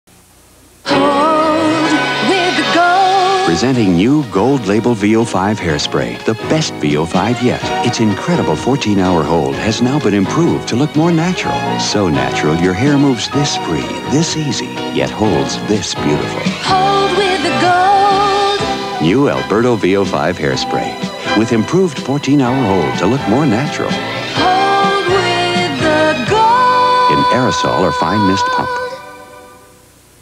Alberto VO5 Hairspray Commercial 1983 (Horse Rider)
I while back I posted a Silkience Hairspray commercial that used slow motion to show the ladie's hair in action. Again, it was kinda weird for hairspray products back in the day to use slomo since hairspray was intended to prevent hair from moving around. But if Silkience can do it, why not Alberto?

Here is a commercial for the new "Gold Label" VO5 hairspray (Hold with the Gold). I loved this ad. The woman riding on the horse was so pretty, and her hair looked amazing bouncing in slow motion (why didn't Pert shampoo do something like this). Enjoy.